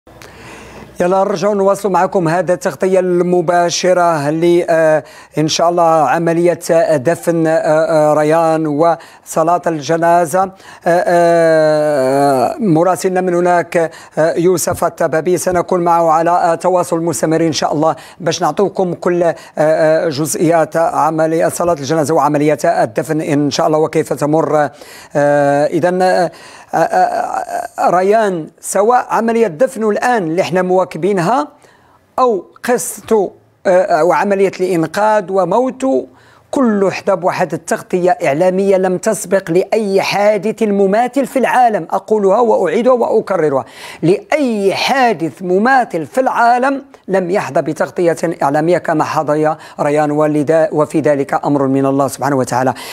حسين شهب : قصة الطفل ريان حظيت بتغطية إعلامية لم تسبق لأي حادث في العالم
CHADA TV | حسين شهب : قصة الطفل ريان حظيت بتغطية إعلامية لم تسبق لأي حادث في العالم

1ère Chaîne Artistique au Maroc